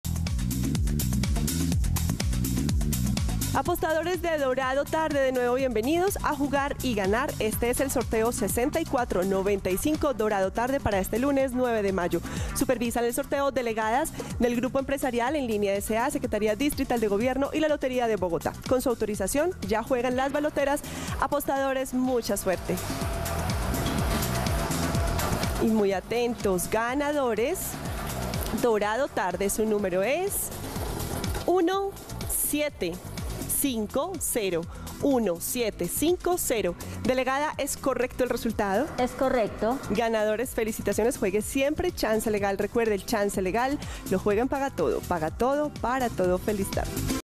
EL DORADO TARDE Resultado del LUNES 09 DE MAYO de 2022 ✅🥇🔥💰
Hola buenas tardes, te presentamos los resultados de las loterías y chances en Colombia, vamos con el Resultados del Chance Dorado tarde del LUNES 09 DE MAYO de 2022.